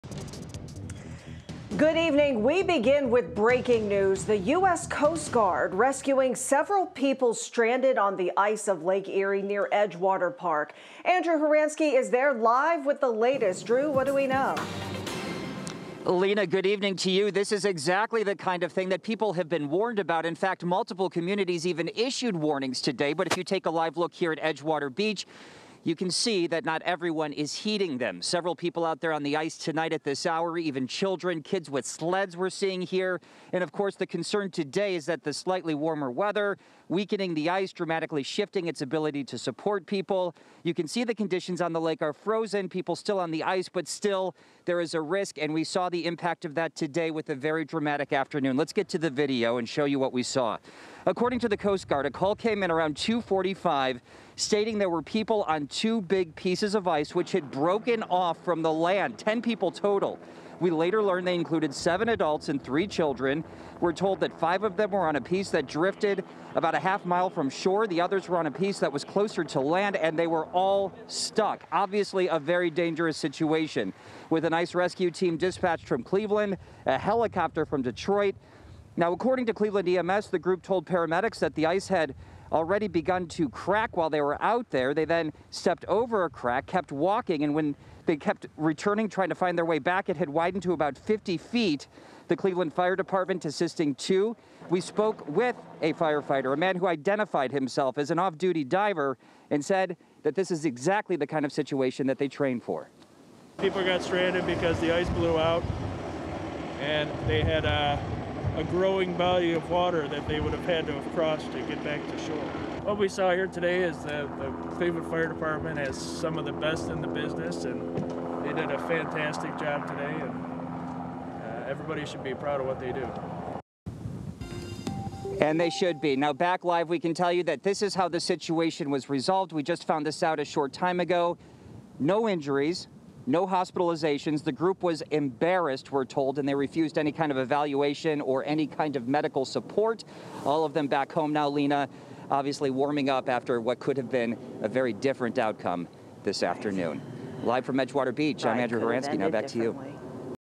U.S. Coast Guard rescues 10 people on the coast of Lake Erie at Edgewater Park
The Coast Guard first received reports around 2:45 p.m. on Sunday. The Coast Guard has rescued all 10 people from Lake Erie.